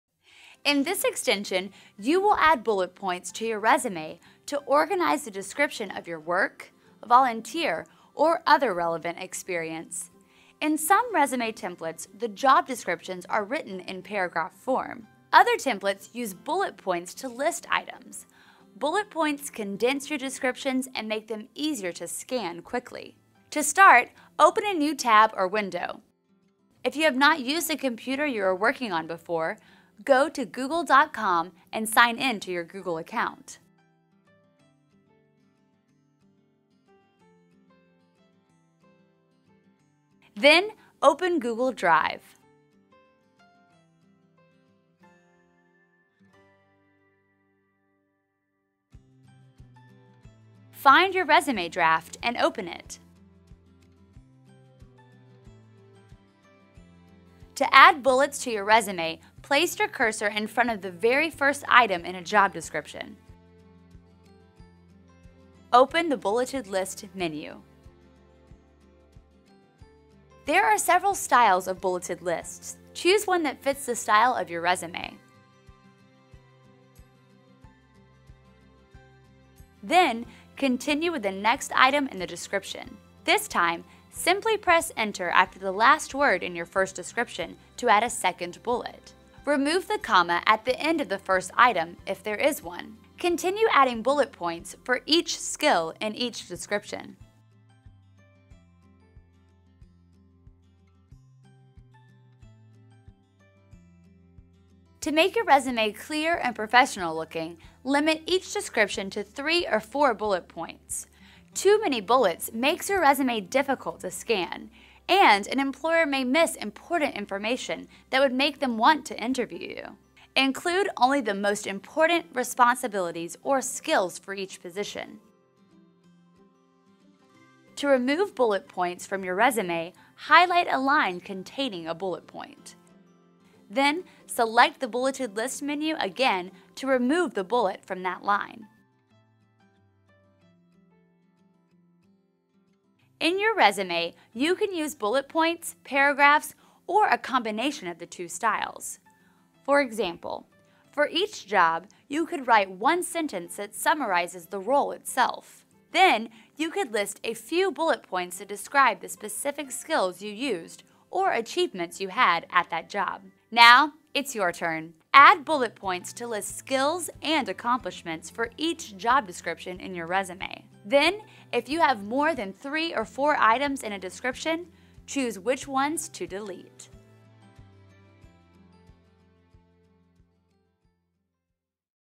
Add Bullet Points to Your Resume
In this extension video, you'll learn why and how to use bullet points to organize information on your resume.

0:00 - Why should you use bullet points in your work descriptions?
0:23 - Sign in to your Google account
0:40 - Open Google Drive, find your resume draft and open it
0:57 - How to add bullet points to a job description
1:45 - How to write a clear and professional description with bullet points
2:23 - How else can you use bullet points in a resume?
2:44 - Recap: Add bullet points to your resume